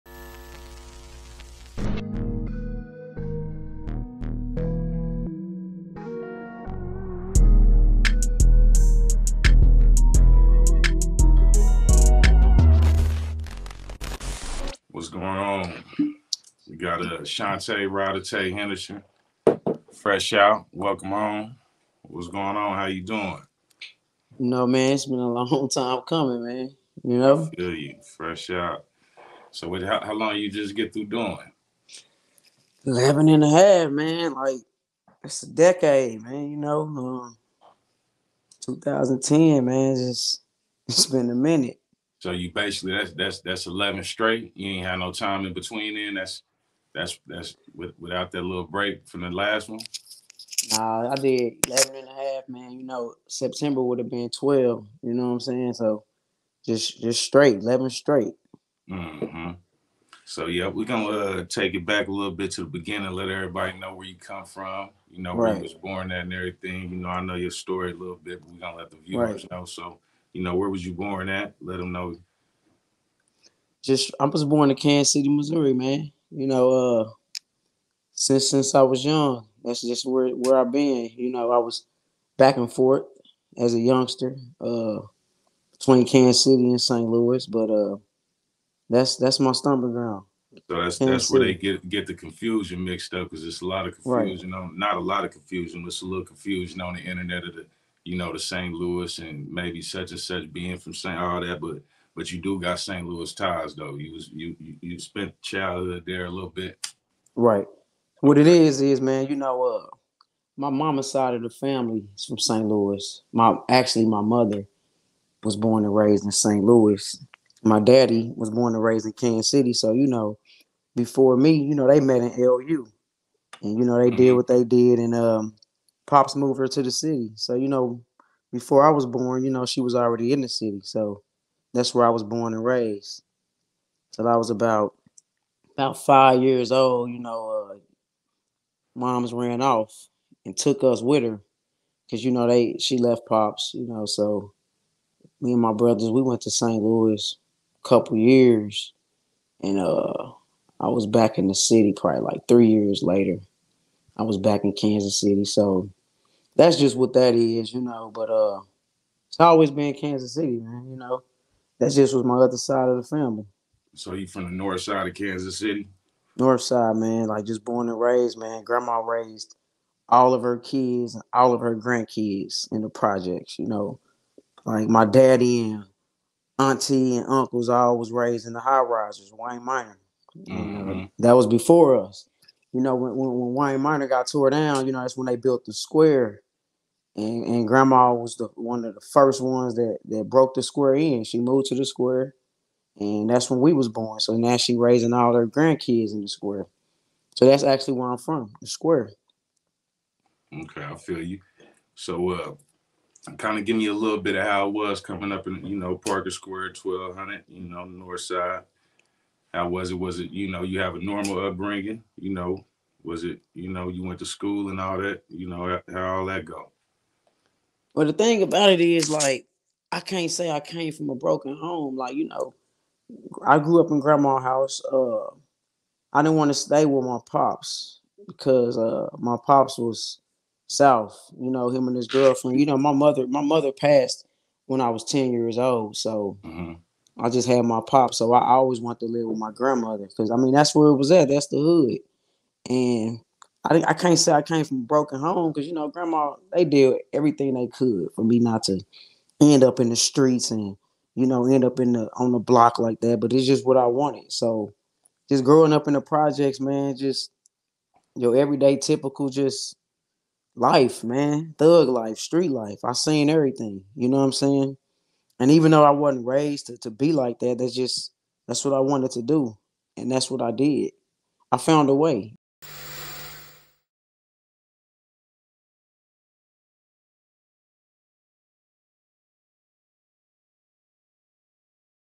Shauntay ''Rydah Tay'' Henderson, Growing up in the projects and early childhood (Part 1)
After serving 11 years Shauntay ''Rydah Tay'' Henderson gives 1st interview and talks about early childhood and growing up in the projects of Kansas City Mo